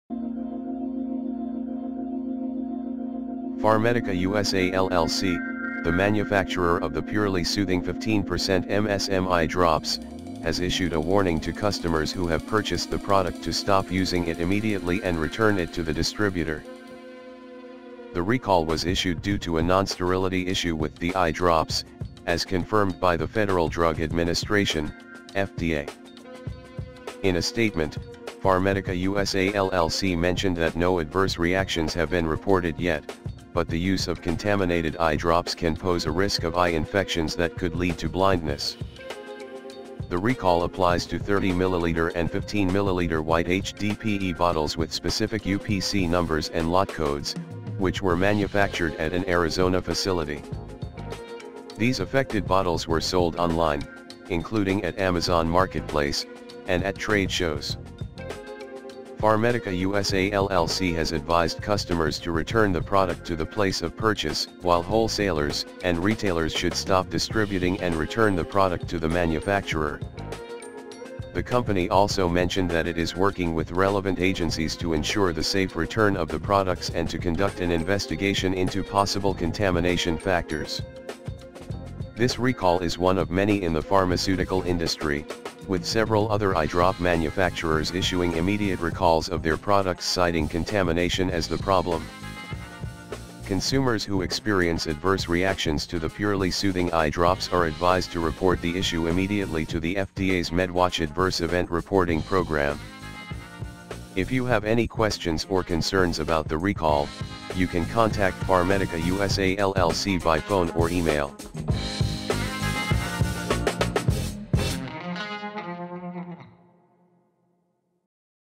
Important Recall Notice Purely Soothing 15% MSM Eye Drops What You Need to Know
If you have purchased the Pharmedica Purely Soothing 15% MSM eye drops, you need to stop using them immediately and return them to the distributor. Pharmedica has issued a recall in compliance with the FDA due to a non-sterility issue with the eye drops. Although there have been no reported adverse reactions yet, the use of contaminated eye drops can result in eye infections that could lead to blindness. The recall applies to certain bottles with specific UPC and lot codes, and these bottles were sold online and at trade shows. Pharmedica is advising customers to return the product and stop using it immediately, while wholesalers and retailers should stop distributing or return the product to Pharmedica. This recall comes amidst several other eye drop recalls due to contamination issues in the pharmaceutical industry. If you experience any adverse reactions to the Purely Soothing eye drops, report the issue immediately to the FDA’s MedWatch Adverse Event Reporting program online or by mail. Contact Pharmedica if you have any questions or concerns. Stay safe and informed with this important recall alert!